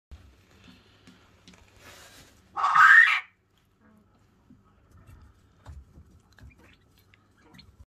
Wachtel Hahn Kräht / Quail rooster crows Sound of male quail
Unser 6 Wochen junger Hahn kräht